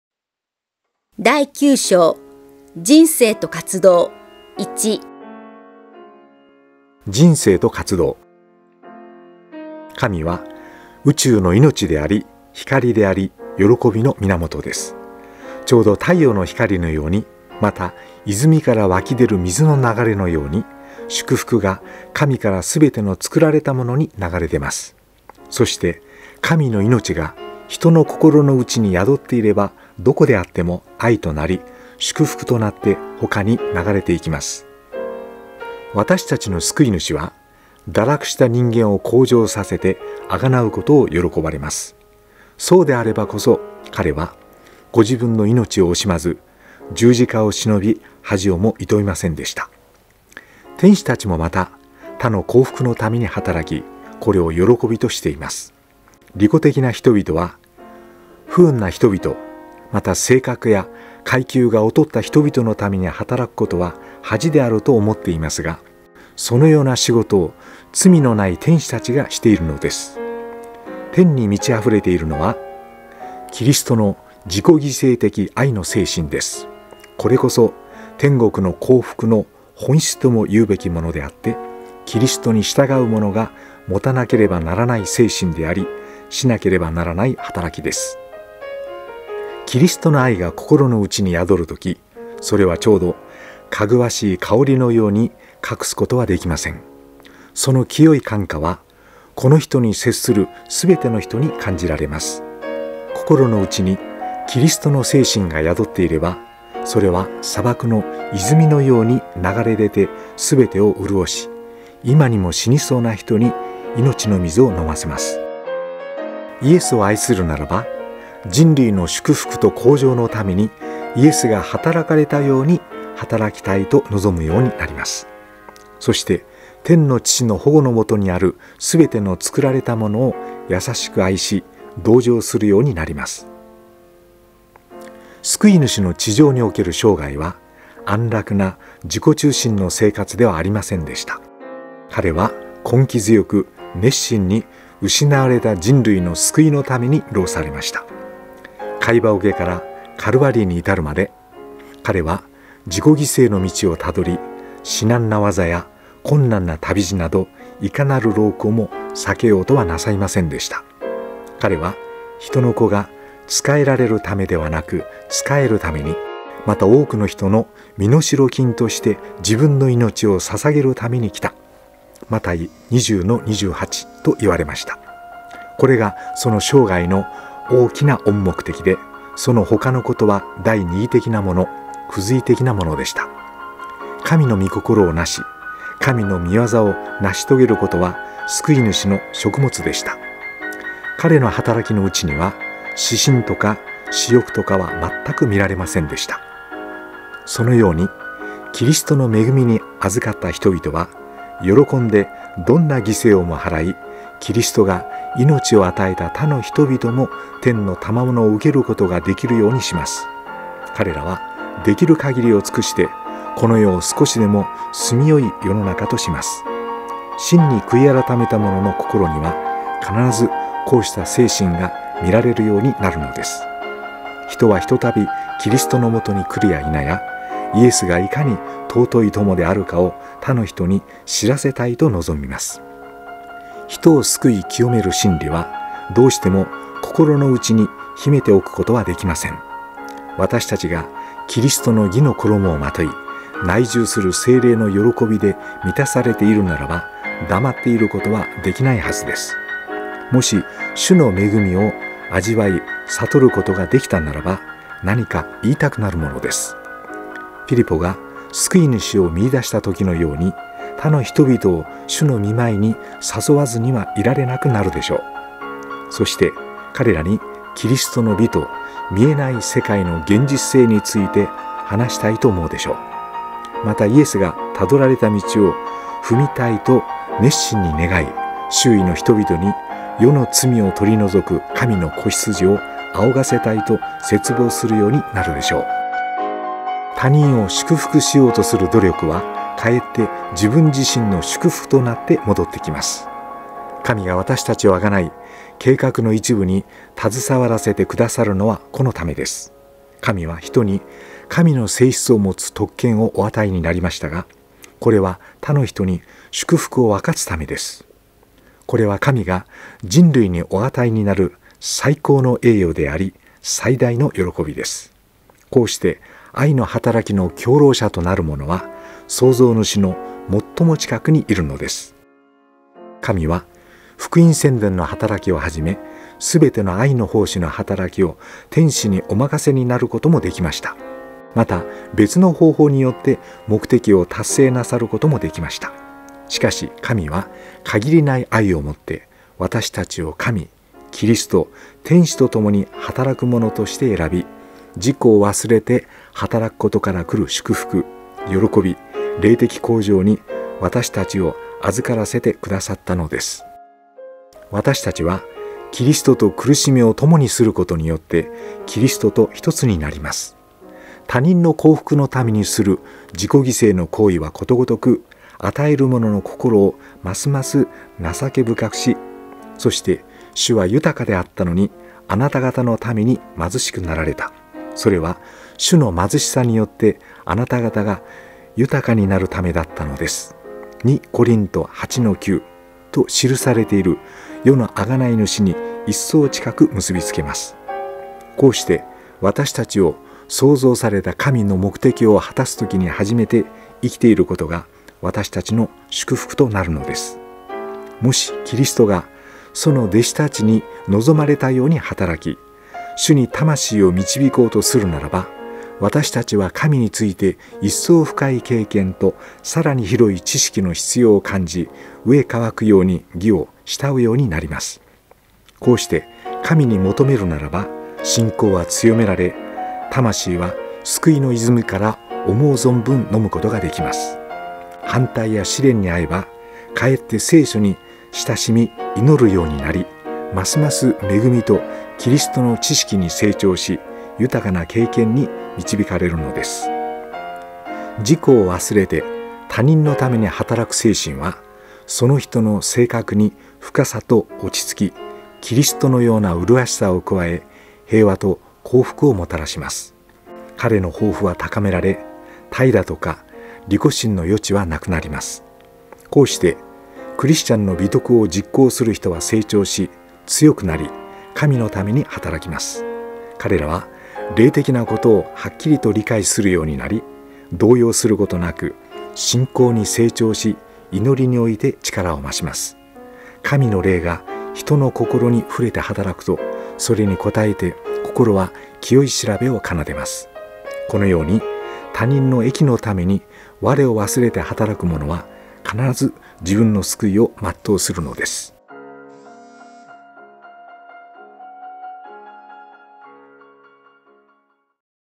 キリストへの道 3 第九章 人生と活動 -1-【字幕付き】
川越勝牧師が朗読する『キリストへの道』

朗読・川越勝
2012年7月15日製作
CD4枚組
2,200円（税込）

注：元のCDはBGMと字幕は　ございません